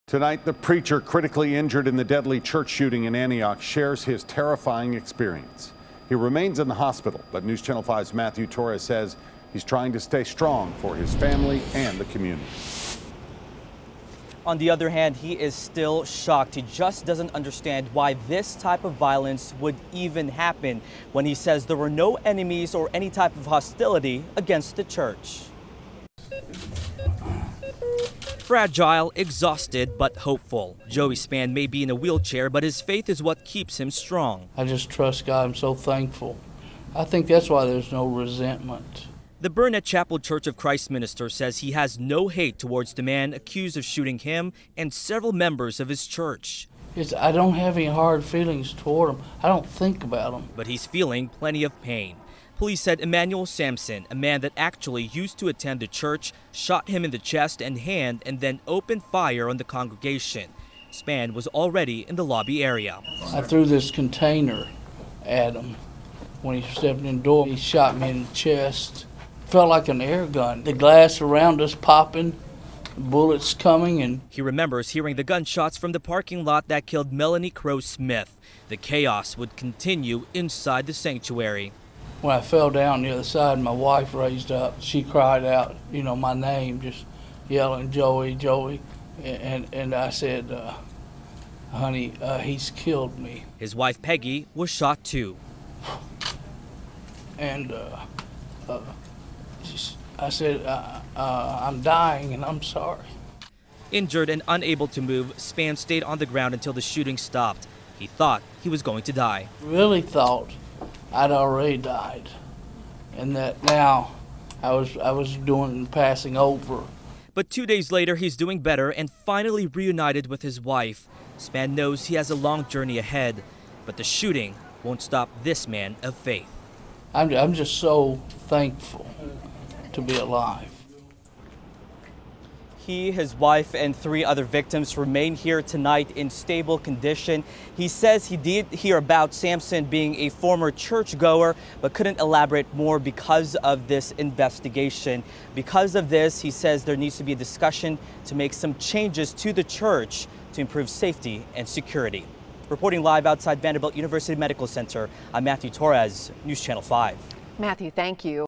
Minister Describes Chaos Of Antioch Church Shooting
The minister injured in Sundays shooting at an Antioch church spoke publicly about the chaotic moments when agunmen entered and opened fire.